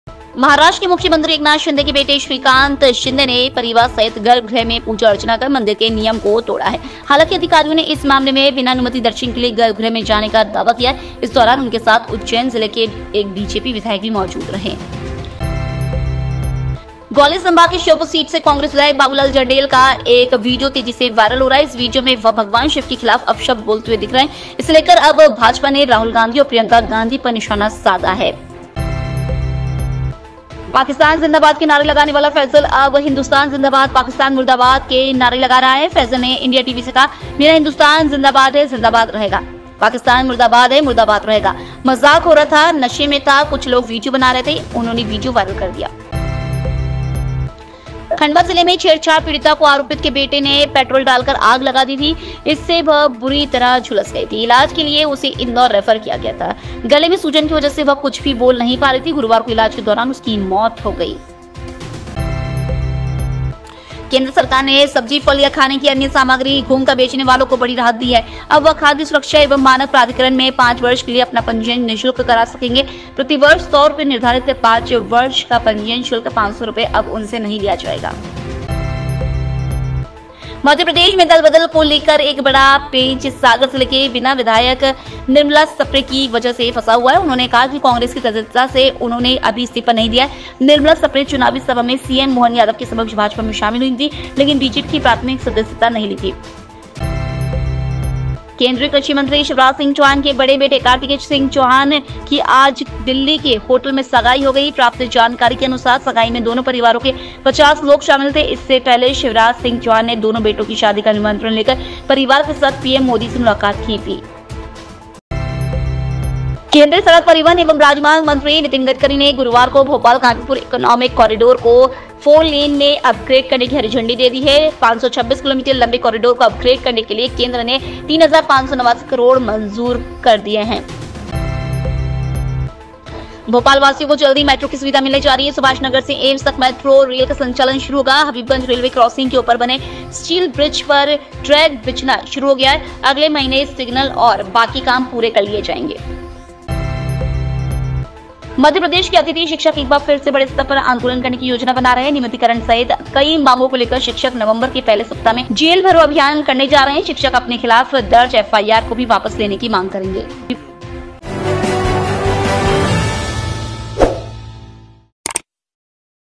सीएम के बेटे ने तोड़े महाकाल मंदिर के नियम | EMS TV 18-Oct-2024
maharashtranews eknathshinde ujjainnews महाराष्ट्र के मुख्यमंत्री एकनाथ शिंदे के बेटे श्रीकांत शिंदे ने परिवार सहित गर्भगृह में पूजा अर्चना कर मंदिर के नियम को तोड़ा है. हालांकि, अधिकारियों ने इस मामले में बिना अनुमति दर्शन के लिए गर्भगृह में जाने का दावा किया है. इस दौरान उनके साथ उज्जैन जिले के एक बीजेपी विधायक भी मौजूद रहे.

CongressMLA said abusive words against Lord Shiva

congress MLABabulalJandel from Sheopur seat of Gwalior division. का एक वीडियो तेजी से वायरल हो रहा है। इस वीडियो में वह भगवान शिव के खिलाफ अपशब्द बोलते दिख रहे हैं। इसे लेकर अब भाजपा ने राहुल गांदी और प्रियंका गांधी पर निशाना साधा है। उन्होंने कहा,यह कांग्रेस की परिपाटी है और पद्धति भी है।

मुझे देश से प्यार नशे में बनाया विडियो
पाकिस्तान जिंदाबाद के नारे लगाने वाला फैजल अब हिंदुस्तान जिंदाबाद पाकिस्तान मुर्दाबाद के नारे लगा रहा है। फैजल ने इंडिया टीवी से कहा "मेरा हिंदुस्तान जिंदाबाद है, जिंदाबाद रहेगा। पाकिस्तन मुर्दाबाद है, मुर्दाबाद रहेगा। मजाक हो रहा था नशे में था, कुछ लोग वीडियो बना रहे थे, उन्होंने वीडियो वायरल कर दिया।

छेड़छाड़ पीड़‍िता पर पेट्रोल डालकर लगा दी आग

खंडवा जिले में छेड़छाड़ पीड़‍िता को आरोपित के बेटे ने पेट्रोल डालकर आग लगा दी थी। इससे वह बुरी तरह झुलस गई थी। इलाज के लिए उसे इंदौर रेफर किया गया था। गले में सूजन की वजह से वह कुछ भी बोल नहीं पा रही थी। गुरुवार को इलाज के दौरान उसकी मौत हो गई।

मध्‍य प्रदेश में फल, सब्जी का मुफ्त पंजीयन

केंद्र सरकार ने सब्जी-फल या खाने की अन्य सामग्री घूमकर बेचने वालों को बड़ी राहत दी है। अब वह खाद्य सुरक्षा एवं मानक प्राधिकरण (एफएसएसएआइ) में पांच वर्ष के लिए अपना पंजीयन निशुल्क करा सकेंगे। प्रतिवर्ष सौ रुपये निर्धारित पांच वर्ष का पंजीयन शुल्क 500 रुपये अब उनसे नहीं लिया जाएगा।

बीना विधायक निर्मला सप्रे बीजेपी में हैं या कांग्रेस में?

मध्य प्रदेश में दलबदल को लेकर एक बड़ा पेंच सागर जिले के बीना विधायक निर्मला सप्रे की वजह से फंसा हुआ है. उन्होंने कहा कि कांग्रेस की सदस्यता से उन्होंने अभी इस्तीफा नहीं दिया है. निर्मला सप्रे चुनावी सभा में सीएम मोहन यादव के समक्ष भाजपा में शामिल हुई थी. लेकिन बीजेपी की प्राथमिक सदस्यता नहीं ली थी।
ShivrajSinghChauhan's elder son got engaged in Delhi

केंद्रीय कृषि मंत्री शिवराज सिंह चौहान के बड़े बेटे कार्तिकेय सिंह चौहान की आज (17 अक्टूबर) को दिल्ली के होटल में सगाई हो गई. प्राप्त जानकारी के अनुसार सगाई में दोनों परिवारों के 50 लोग शामिल थे. इससे पहले शिवराज सिंह चौहान ने दोनों बेटों की शादी का निमंत्रण लेकर परिवार के साथ पीएम मोदी से मुलाकात की थी।

भोपाल कानपुर कॉरिडोर को केंद्र की हरी झंडी

केंद्रीय सड़क परिवहन एवं राजमार्ग मंत्री नितिन गडकरी ने गुरुवार(17 अक्टूबर) को भोपाल-कानपुर इकोनॉमिक कॉरिडोर को 4-लेन में अपग्रेड करने की हरी झंडी दे दी है। 526 किमी लंबे कॉरिडोर को अपग्रेड करने के लिए केंद्र ने 3589.4 करोड़ मंजूर कर दिए हैं।

भोपाल वीडियो को जल्द मिलेगी मेट्रो की सवारी

भोपालवासियों को जल्द ही मेट्रो की सुविधा मिलने जा रही है। सुभाष नगर से एम्स तक मेट्रो रेल का संचालन शुरू होगा। हबीबगंज रेलवे क्रॉसिंग के ऊपर बने स्टील ब्रिज पर ट्रैक बिछना शुरू हो गया है। अगले महीने सिग्नल और बाकी काम पूरे कर लिए जाएंगे।

MP के अतिथि शिक्षक फिर करेंगे बड़ा प्रदर्शन

मध्य प्रदेश के अतिथि शिक्षक एक बार फिर बड़े स्तर पर आंदोलन करने की योजना बना रहे हैं। नियमितीकरण सहित कई मांगों को लेकर शिक्षक नवंबर के पहले सप्ताह में जेल भरो अभियान करने जा रहे हैं। शिक्षक अपने खिलाफ दर्ज FIR को भी वापस लेने की मांग करेंगे।